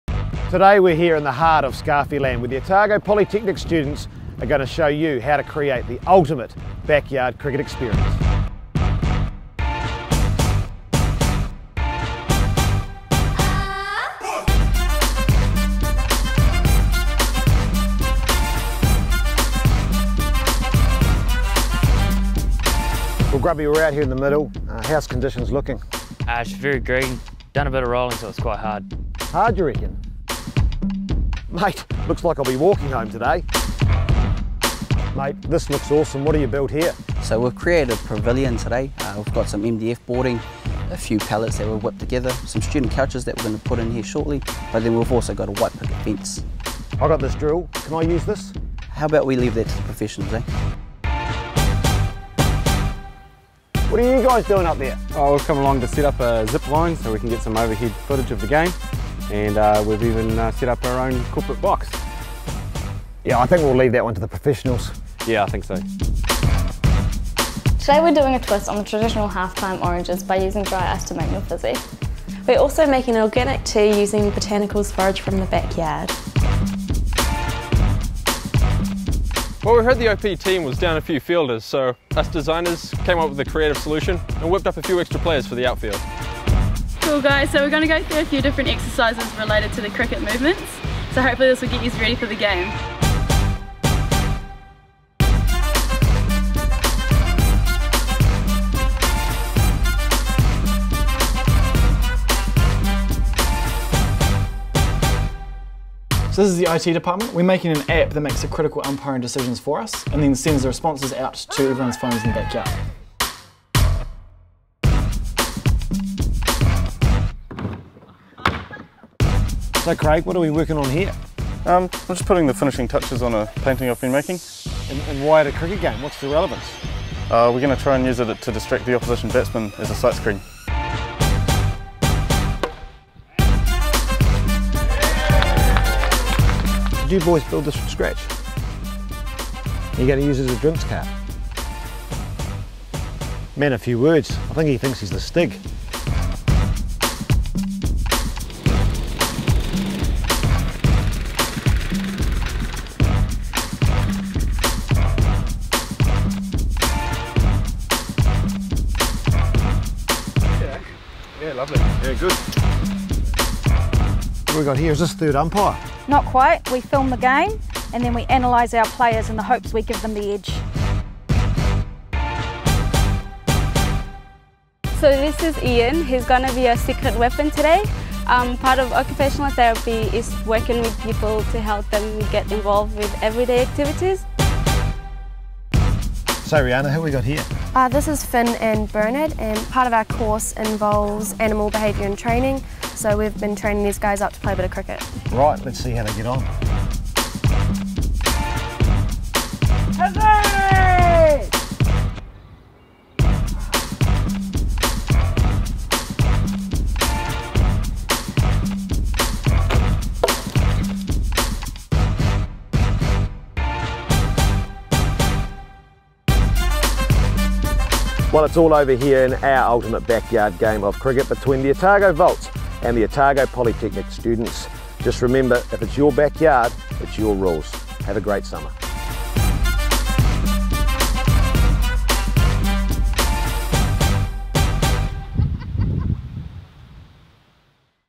How to create the ultimate backyard cricket experience!
Summer is here and like most Kiwi's, we love our cricket! So much so that we challenged the Otago Volts to a game in our own backyard - our rules! Check out this video as our students show you how to create the ultimate backyard cricket experience!